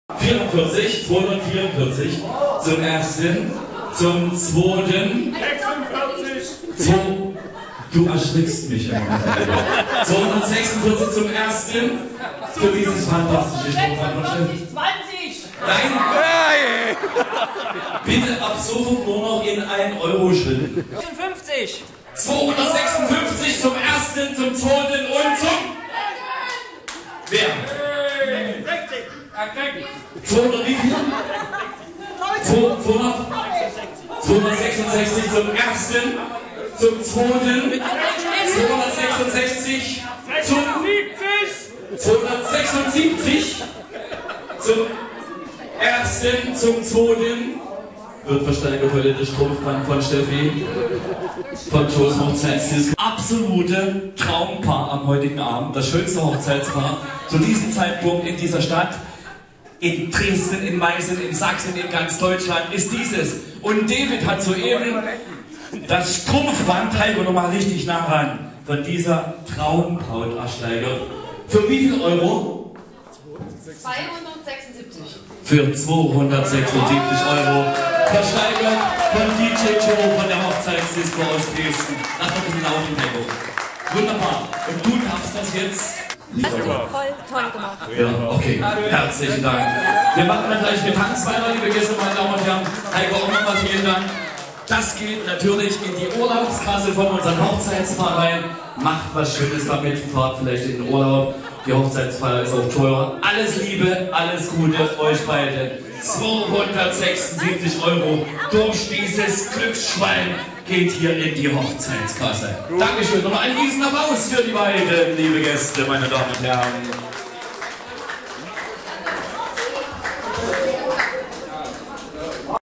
DJ Dresden - Hochzeits DJ Dresden
DJ Joe Moderation Strumpfbandversteigerung für die Braut brachten dem Hochzeitspaar 276 Euro in ihre Urlaubskasse.
https// - DJ Joe buchen unter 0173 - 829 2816